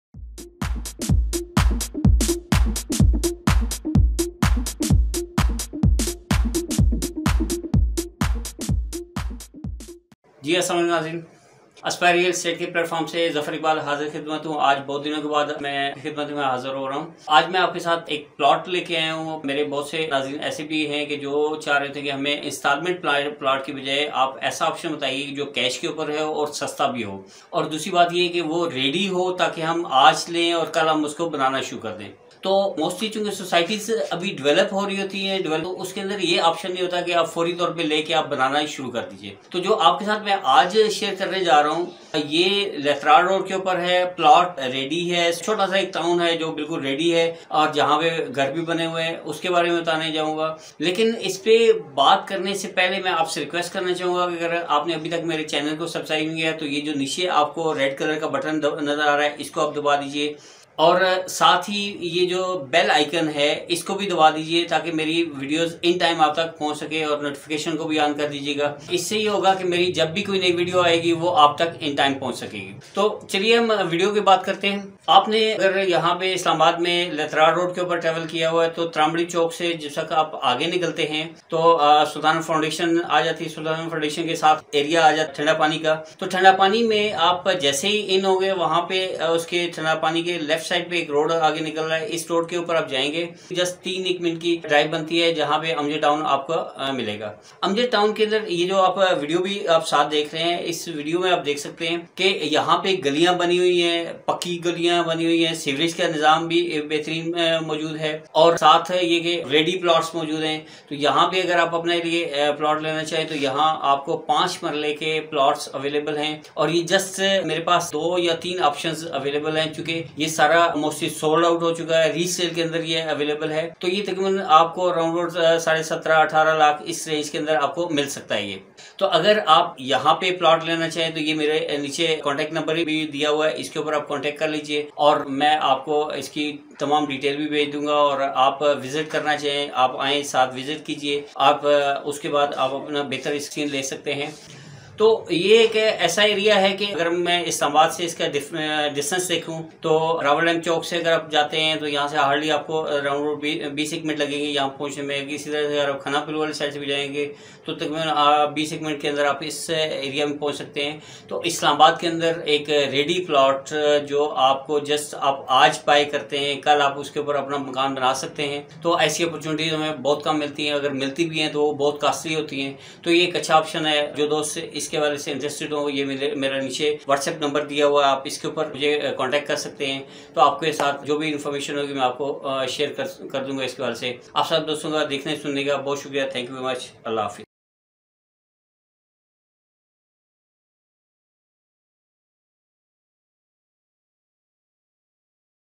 5 Marla Plot for Sale in Islamabad || Thanda Pani Amjad Town
In this video you will fine information regarding 5 Marla Plot for Sale in Islamabad || Thanda Pani Amjad Town Lehtrar Road